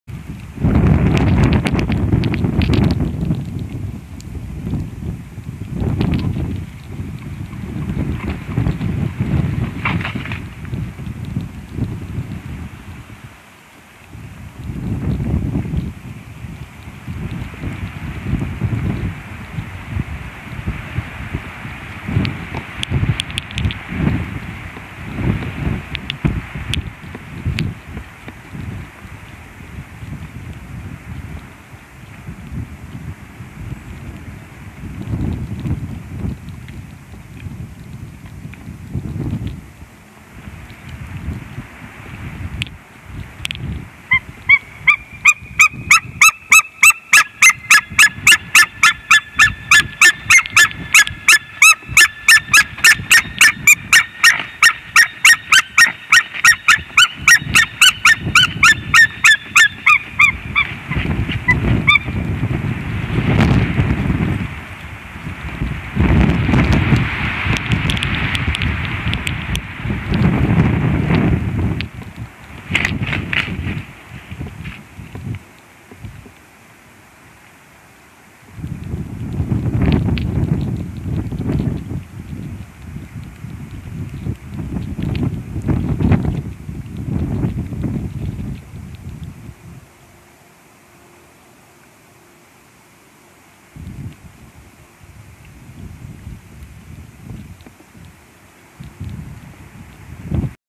Łotwa.. śnieg, a w sercu bielika wiosna 2018 02 07
zalężenie...mimo śniegu i zimy...oby tylko jajka za wcześnie się nie pojawiły ...

LDF Jūras ērglis tiešraide / White-tailed eagle webcam in Latvia